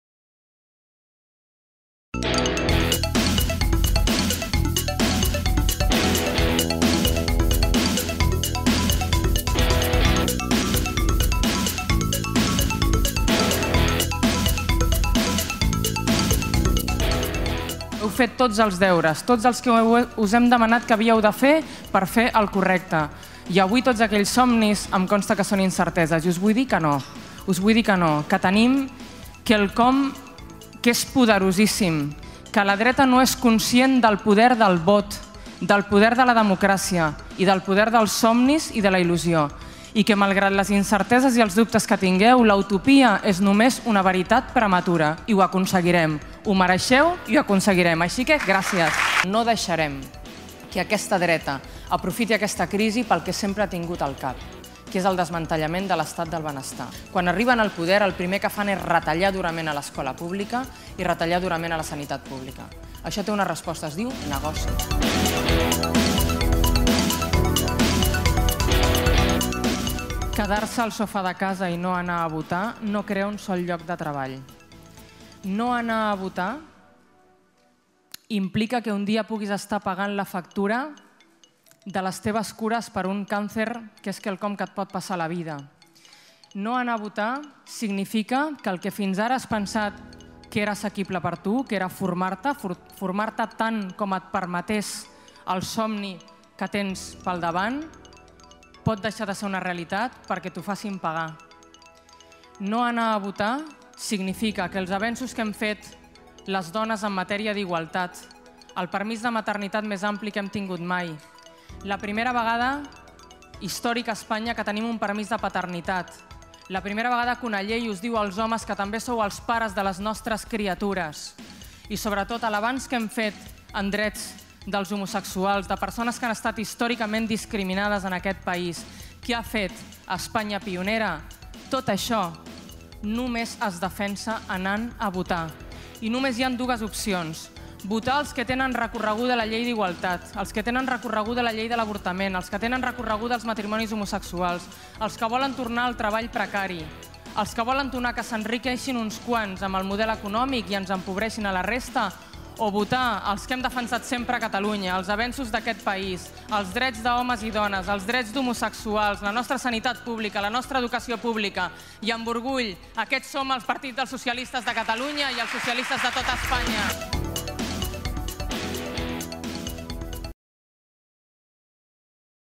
Acte final de campanya de Carme Chacón amb joves
La candidata socialista, Carme Chacón, ha tancat la seva campanya electoral en un acte amb joves de la JSC.